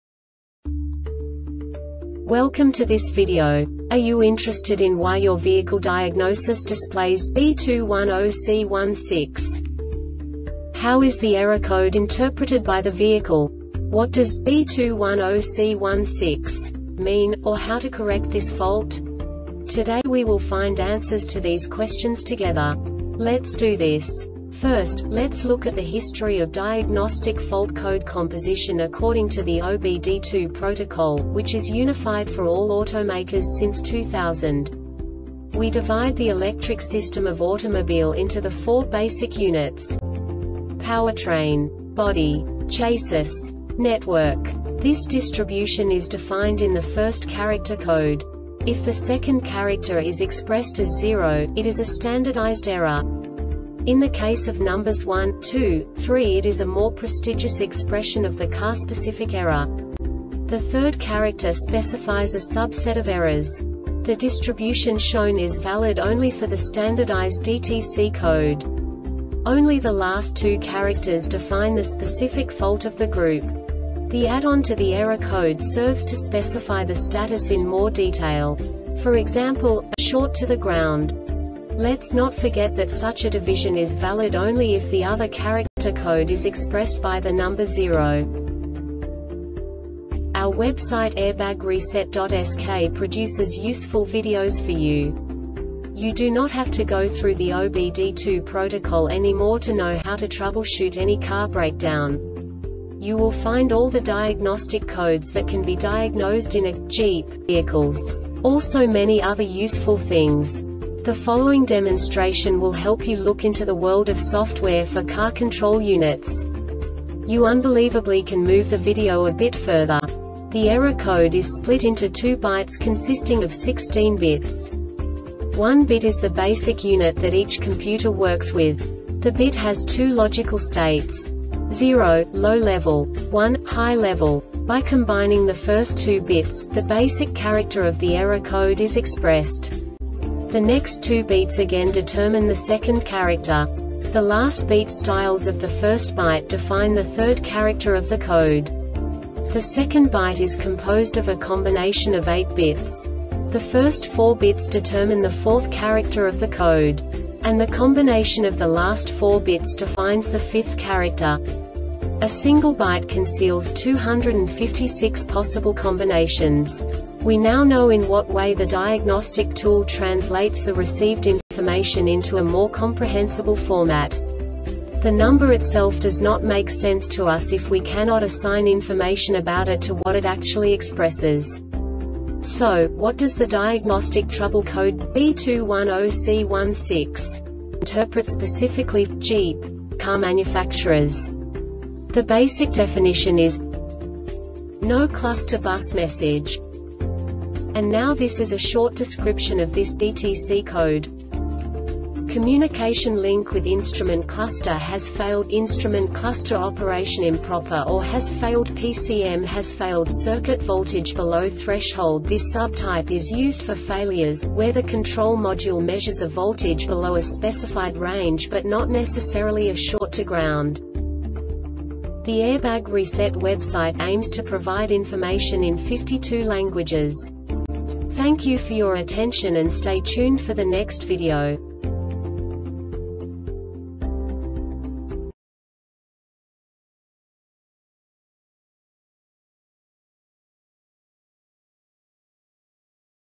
DTC Jeep B210C-16 Short Explanation
Contents:
0:21 Basic DTC analysis according to OBD2 protocol standard.
1:48 Insight into programming
2:58 A diagnostically understandable description of the Diagnostic Trouble Code
3:05 Basic error definition

Make:
Jeep
Code:
B210C 16
Definition:
No Cluster Bus Message
Cause: